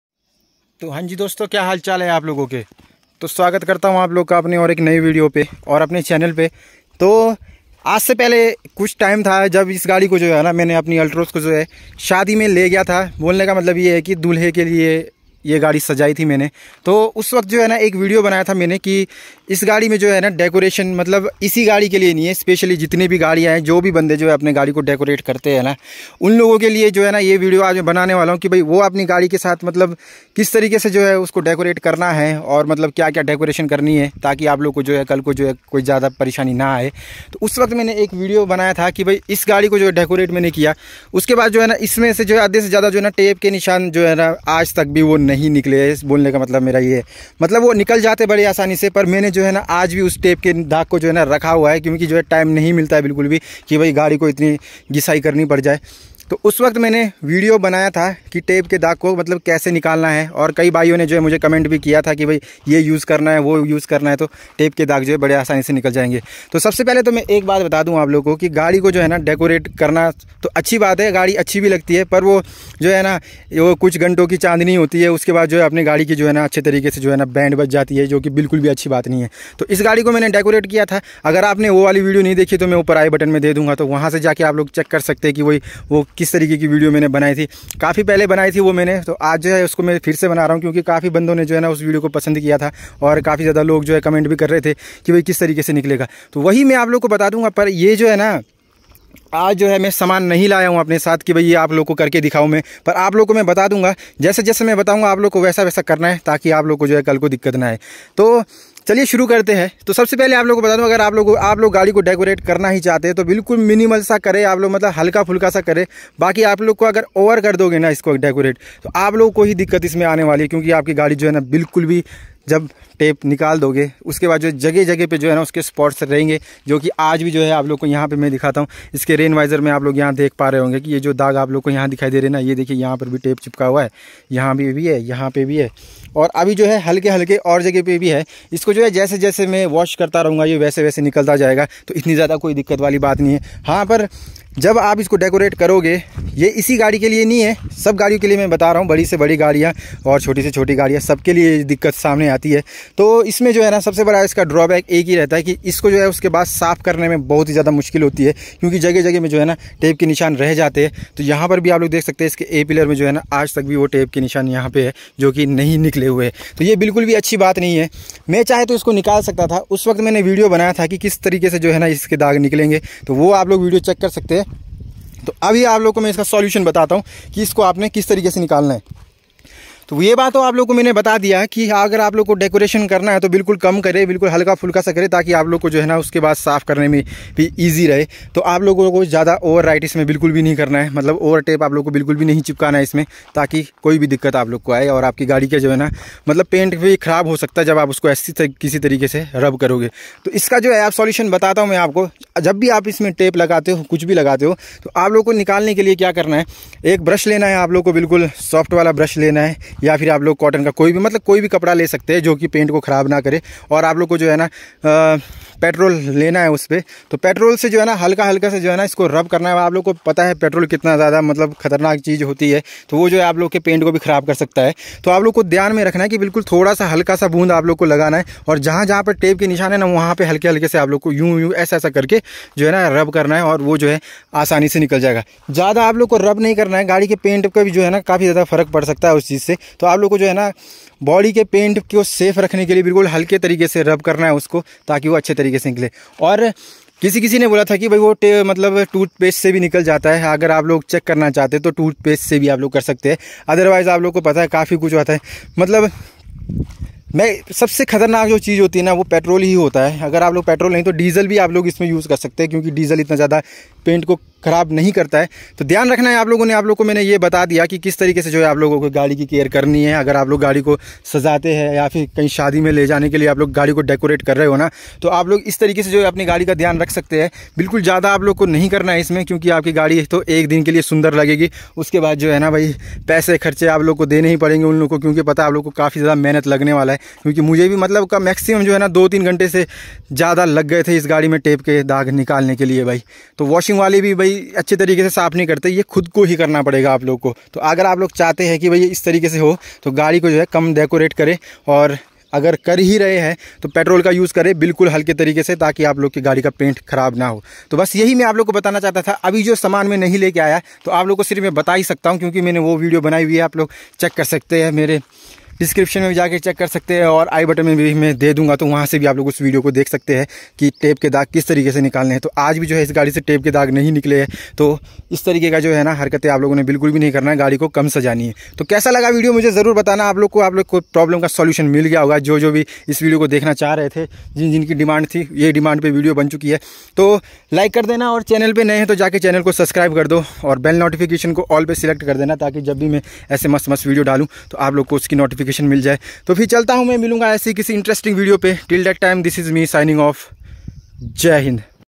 सिर्फ 1 मिनट में अपनी कार पर लगे टेप Spots को हटा दें 🔥 Remove Tape Spots On Your Car In Just 1 mint
To agar apne bhi apni gadi ko decorate kiya hai or ap tape ke daag se pareshan ho to is video ko dheko solution mil jayga and video psnd aay to like krna and channel pe new ho to subscribe kr dena and comment karna na bhule. 🙏🏻🙏🏻🙏🏻🙏🏻

🙏🏻DEV BHUMI HIMACHAL PRADESH🙏🏻